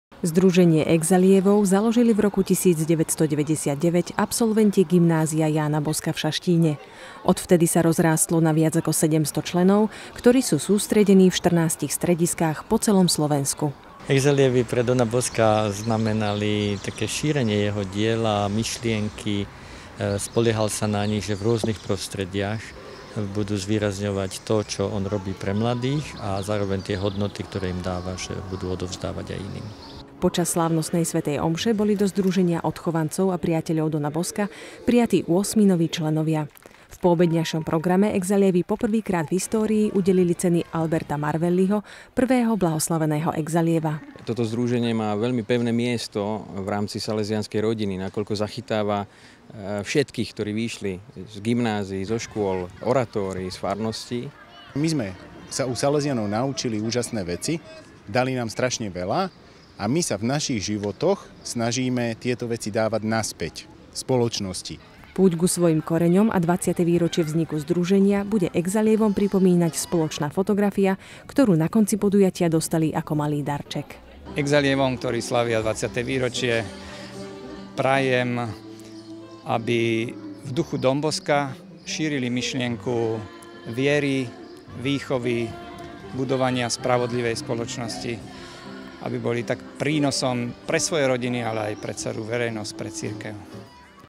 exallievi.sk: 20. výročie Ďakovná púť vláčikom do Šaštína (Reportáž TV Lux)
Oslavy 20. výročia založenia nášho združenia. Mimoriadny vlastný vlak, ďakovná sv. omša s prijímaním nových členov a priateľov dona Bosca, odovzdávanie cien Alberta Marvelliho, spoločný obed, koncert Mira Jila a Aničky Hortovej, stanovištia a súťaže pre deti... Boli ste pri tom aj vy:)?